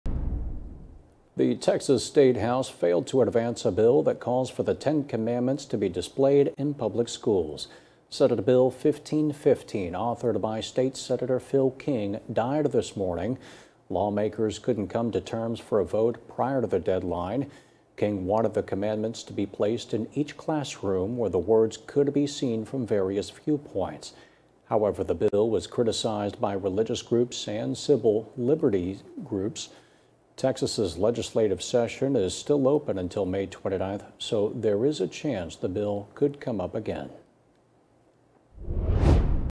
Texas bill requiring schools to display Ten Commandments fails at House
NTV America is available on television as well as Apple TV, Roku, Amazon Fire TV, Android Play, & IOS.